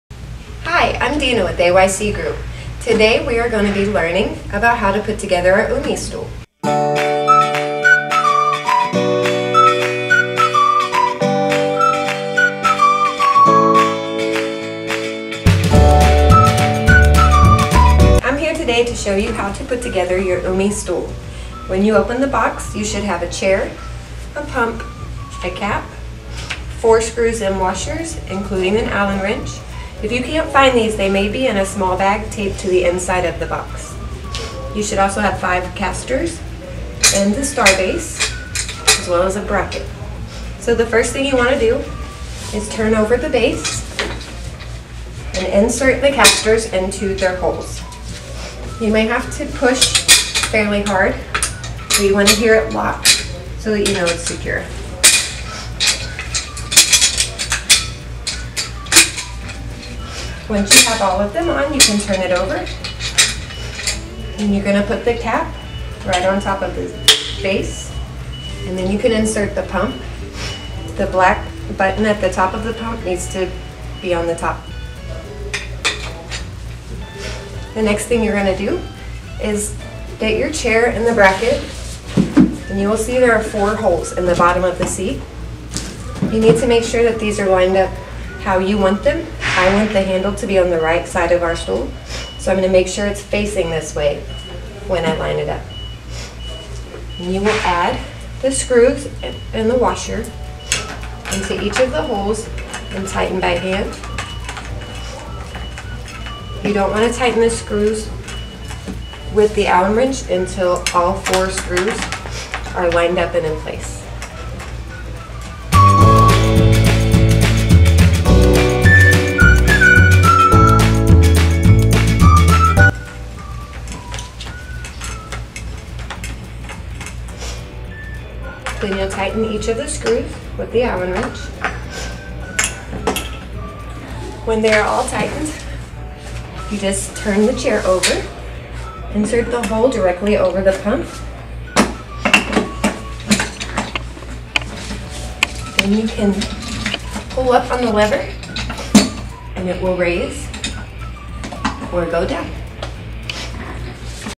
HOW TO: Umi Pedicure Stool
AYC Group's HOW TO Series with the Umi Pedicure Stool. This video is a walkthrough/guidance on how to put together your stool should you need help.